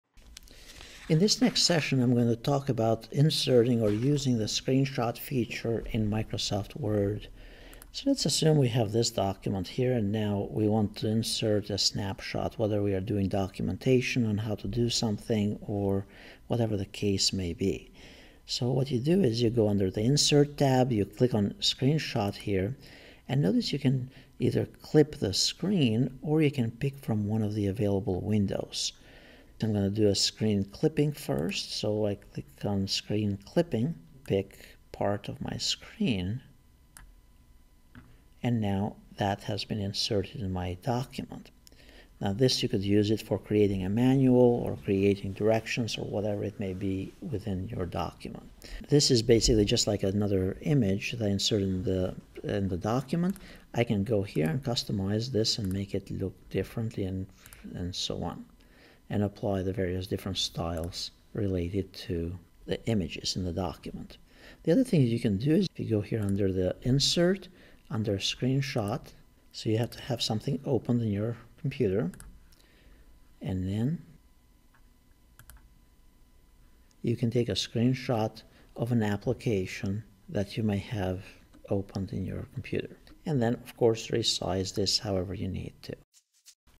Microsoft Word Tutorial - Screen Clipping
Learn how to use screen clipping in Microsoft Word for Microsoft 365, Office 365.